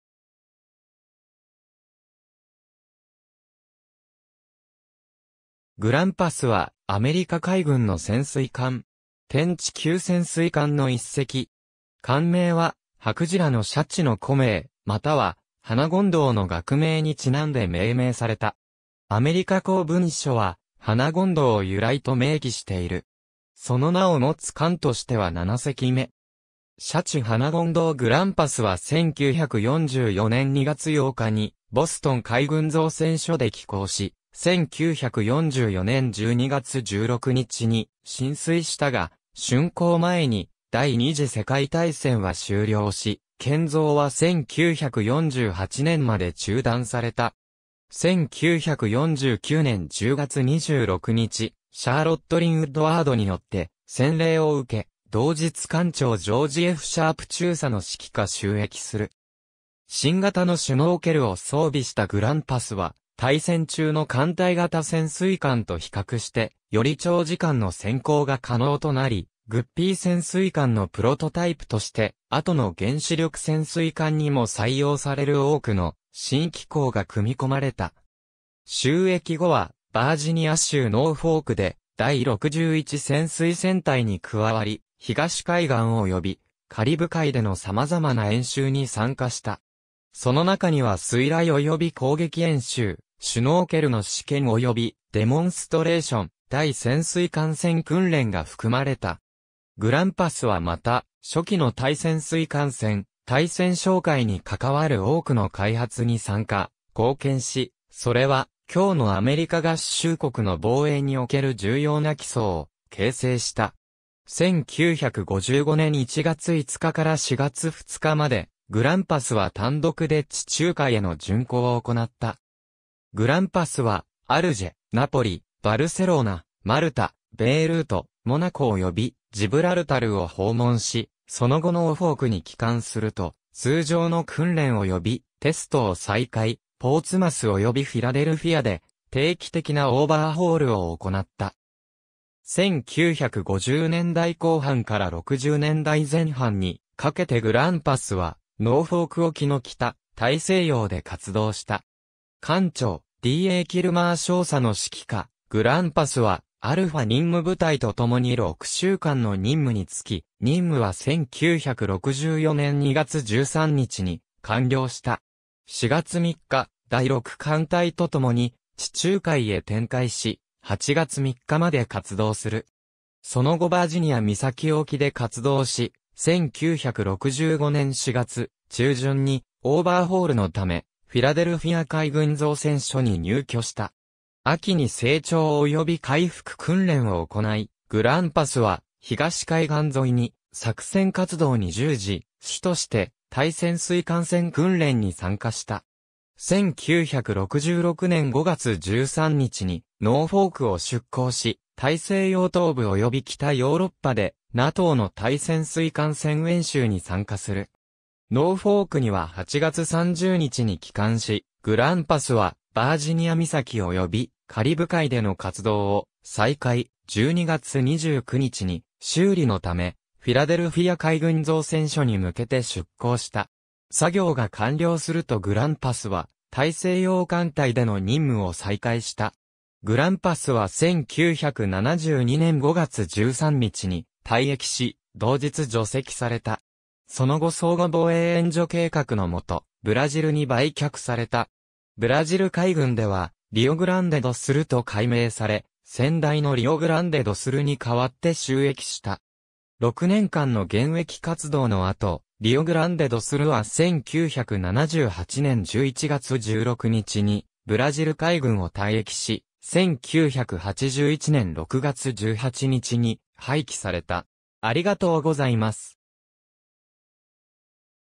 グランパス (SS-523)
グランパス (USS Grampus, SS-523) は、アメリカ海軍の潜水艦。
テンチ級潜水艦の一隻。
艦名は歯クジラのシャチの古名またはハナゴンドウの学名に因んで命名された。
アメリカ公文書はハナゴンドウを由来と明記している。
シャチ（古名Grampus、現代口語はKiller whale） ハナゴンドウ（学名Grampus griseus） グランパスは1944年2月8日にボストン海軍造船所で起工し、1944年12月16日に進水したが、竣工前に第二次世界大戦は終了し建造は1948年まで中断された。
1949年10月26日、シャーロット・リン・ウッドワード（クラーク・H・ウッドワード少将の妻。彼女は先代のグランパスの洗礼も行った。）によって洗礼を受け、同日艦長ジョージ・F・シャープ中佐の指揮下就役する。
新型のシュノーケルを装備したグランパスは、大戦中の艦隊型潜水艦と比較してより長時間の潜航が可能となり、 GUPPY 潜水艦のプロトタイプとして後の原子力潜水艦にも採用される多くの新機構が組み込まれた。
就役後はバージニア州ノーフォークで第61潜水戦隊に加わり、東海岸およびカリブ海での様々な演習に参加した。
その中には水雷および攻撃演習、シュノーケルの試験およびデモンストレーション、対潜水艦戦訓練が含まれた。
グランパスはまた、初期の対潜水艦戦、対潜哨戒に関わる多くの開発に参加、貢献し、それは今日のアメリカ合衆国の防衛における重要な基礎を形成した。
1955年1月5日から4月2日までグランパスは単独で地中海への巡航を行った。
グランパスはアルジェ、ナポリ、バルセロナ、マルタ、ベイルート、モナコおよびジブラルタルを訪問し、その後ノーフォークに帰還すると通常の訓練およびテストを再開、ポーツマスおよびフィラデルフィアで定期的なオーバーホールを行った。
1950年代後半から60年代前半にかけてグランパスはノーフォーク沖の北大西洋で活動した。
艦長D・A・キルマー少佐の指揮下、グランパスは「アルファ」任務部隊と共に6週間の任務に就き、任務は1964年2月13日に完了した。
4月3日、第6艦隊と共に地中海へ展開し、8月3日まで活動する。
その後バージニア岬沖で活動し、1965年4月中旬にオーバーホールのためフィラデルフィア海軍造船所に入渠した。
秋に整調および回復訓練を行い、グランパスは東海岸沿いに作戦活動に従事、主として対潜水艦戦訓練に参加した。
1966年5月13日にノーフォークを出航し、大西洋東部および北ヨーロッパでNATOの対潜水艦戦演習に参加する。
ノーフォークには8月30日に帰還し、グランパスはバージニア岬およびカリブ海での活動を再開、12月29日に修理のためフィラデルフィア海軍造船所に向けて出航した。
作業が完了するとグランパスは大西洋艦隊での任務を再開した。
グランパスは1972年5月13日に退役し、同日除籍された。
その後相互防衛援助計画の下、ブラジルに売却された。
ブラジル海軍ではリオ・グランデ・ド・スル (Rio Grande do Sul, S-11) と改名され、先代のリオ・グランデ・ド・スルに代わって就役した。
6年間の現役活動の後、リオ・グランデ・ド・スルは1978年11月16日にブラジル海軍を退役し、1981年6月18日に廃棄された。